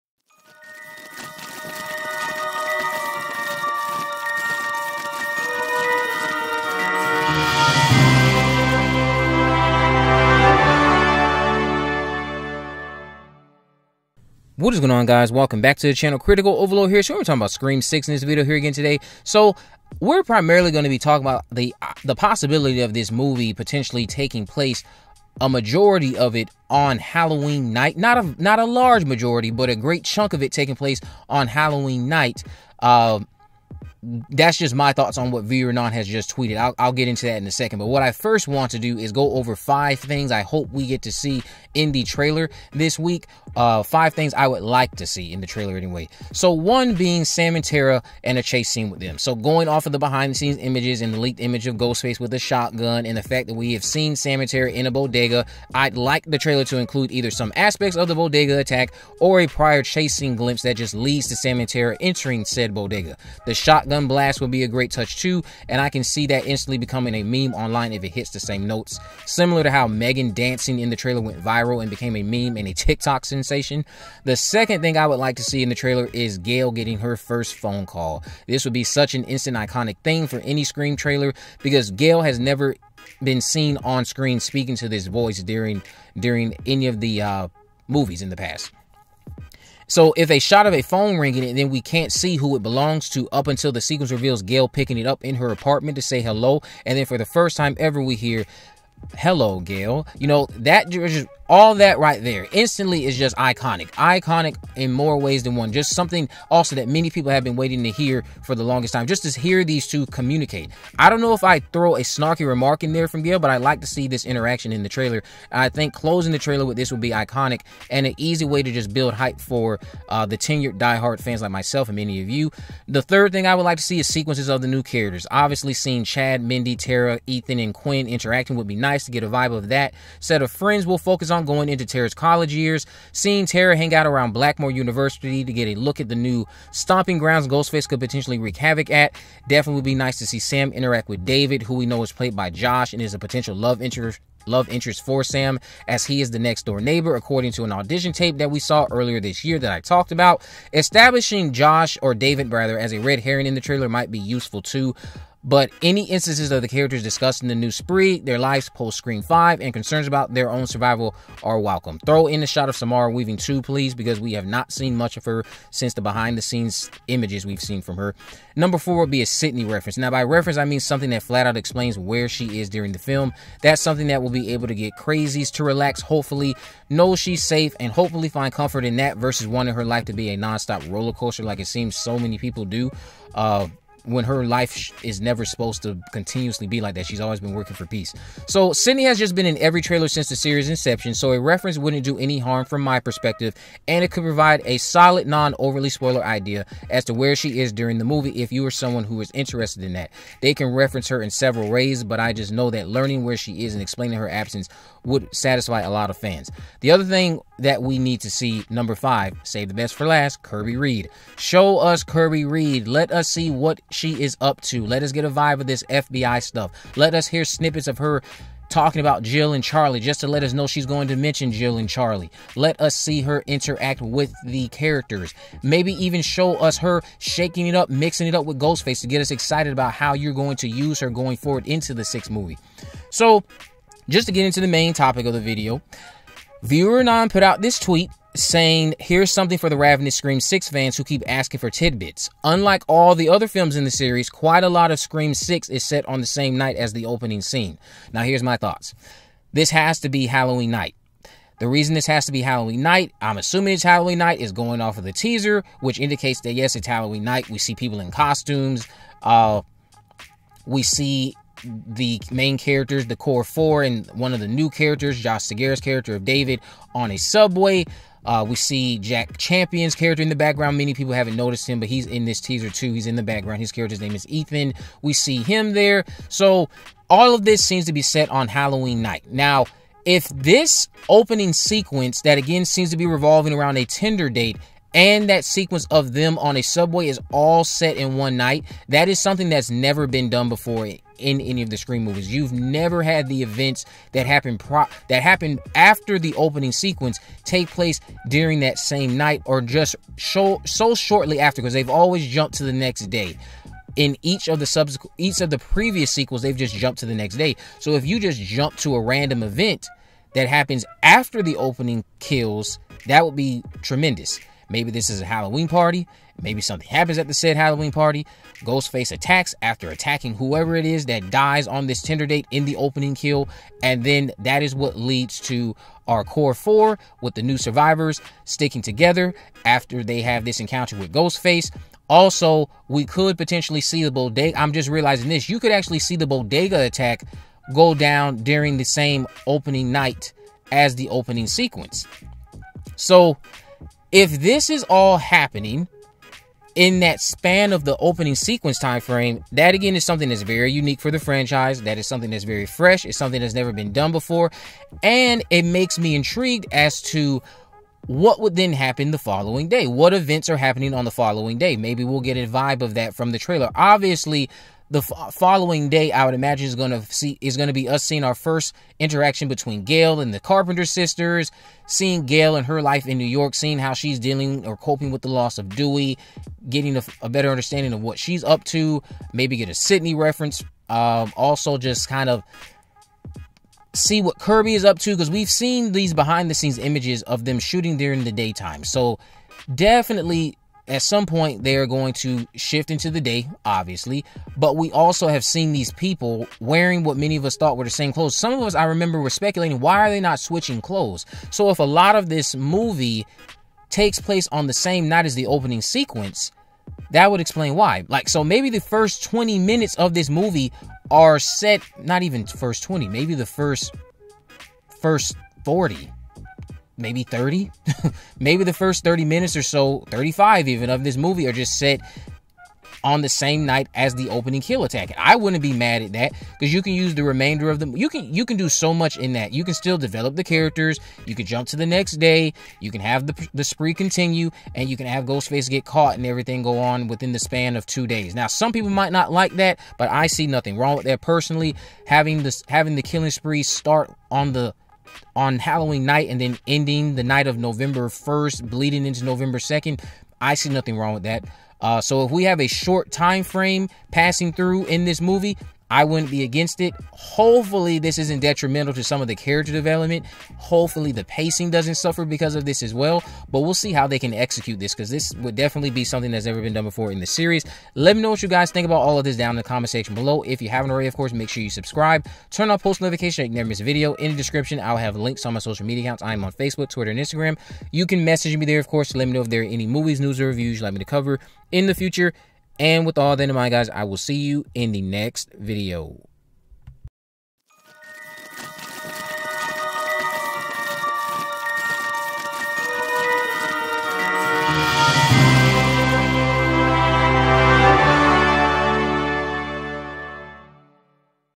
Scream 6 | Ghostface Opening Halloween Attacks?
Scream 6 began filming a few months ago in Montreal and Ghostface's terror in New York has come to a close. Woodsboro will get a break, but the survivors will take Ghostface to their new location. Marketing for Ghostface's New York debut has begun with the first teaser, poster, and new image of Ghostface released. The Opening scene might be an extended set of events taking place on Halloween.

Art By: Samhain1992